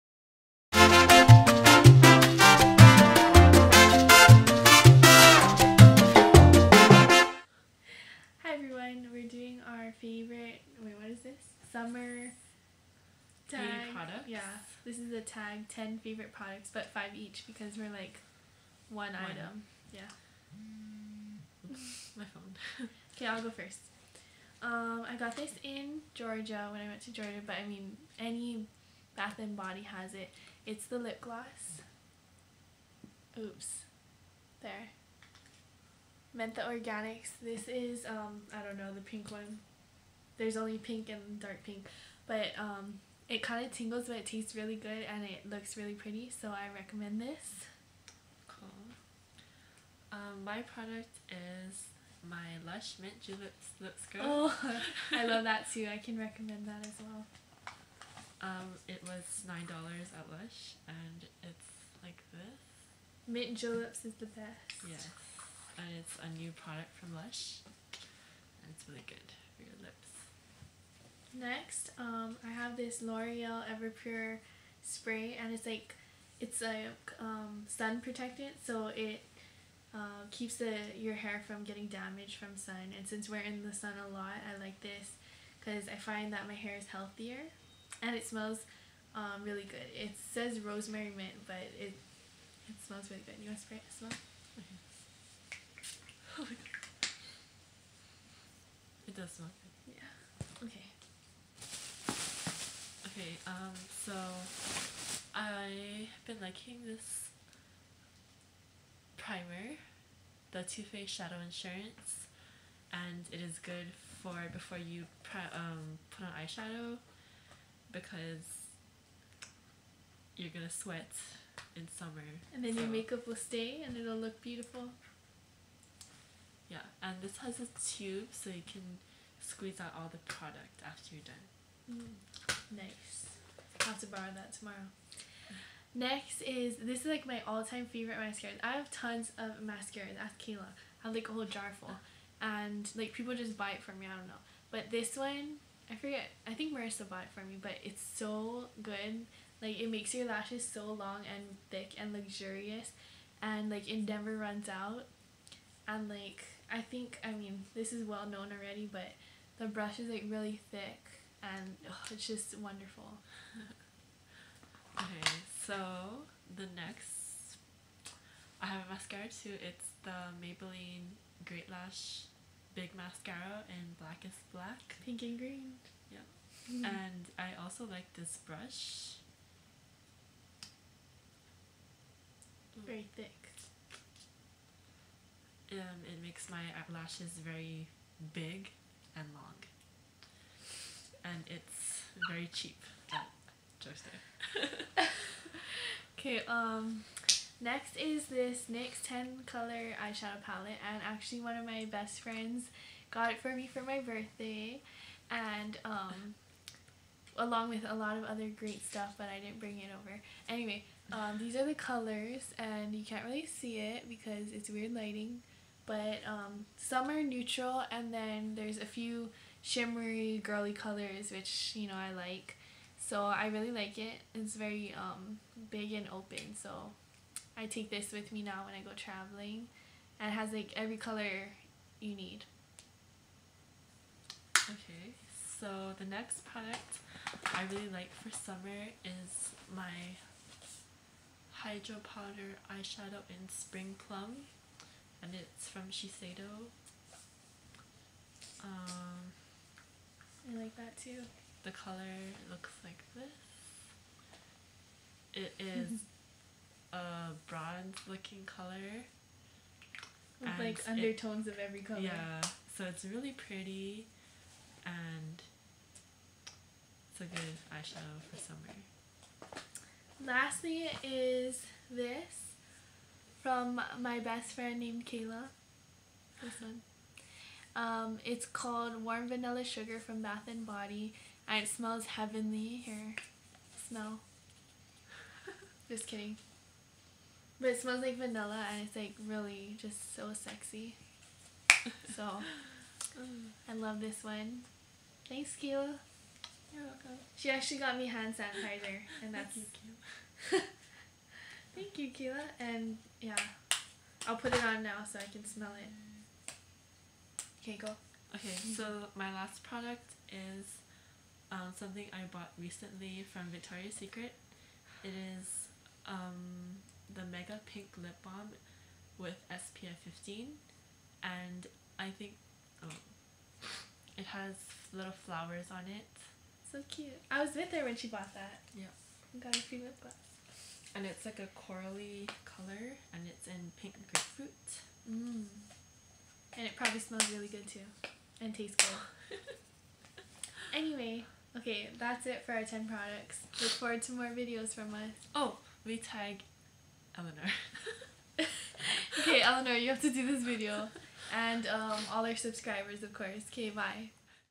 Tag: Top 10 Summer Products
Metha Organics Lip Gloss from Bath and Body Works
Mint Julip Lip Scrub from Lush
L'oreal Ever Pure Spray
Too Faced Shadow Insurance from Sephora
Covergirl LashBlast Mascara
Maybeline Great Lash Mascara in Blackest Black
NYX 10 Color Eye Shadow Palette in Secret World
Sheseido Hydro Powder Eyeshadow in Spring Plum
Bath and Body Works Warm Vanilla Sugar Body Lotion
Victorias Secret PINK lip Balm w/ SPF 15 in Pink Grapefruit

WE TAG. Eleanor!